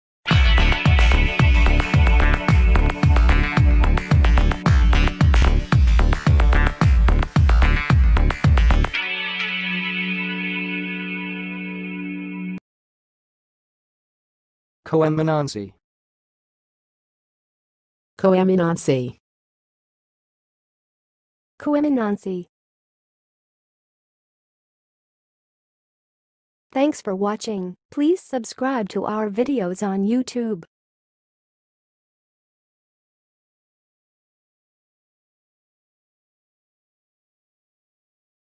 How to say "coeminency"! (High Quality Voices)
Watch in this video how to say and pronounce "coeminency"!